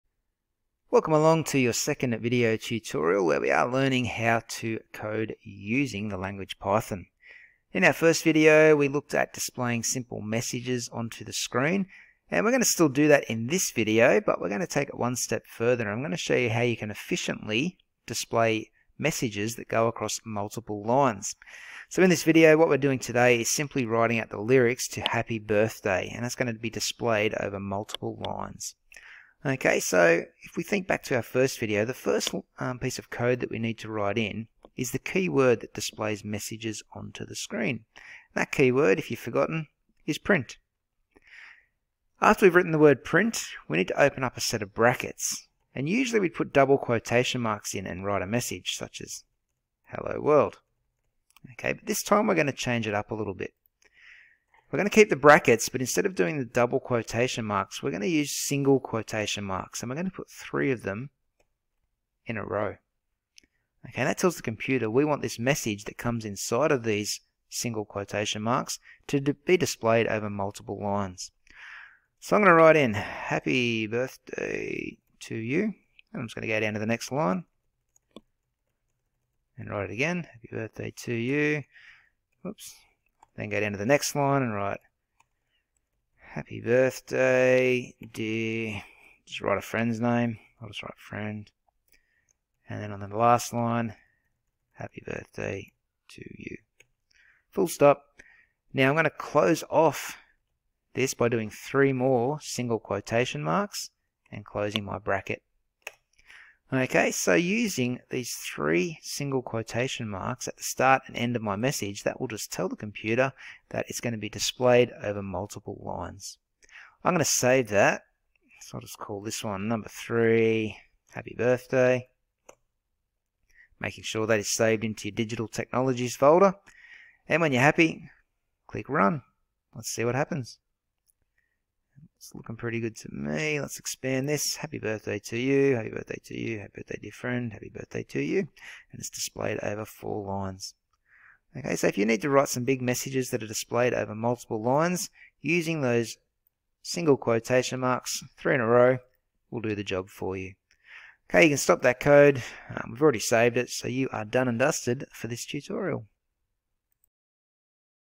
Python - Display Multi-Line Messages Tutorial
Learn how to display longer messages that span over multiple lines using code in Python.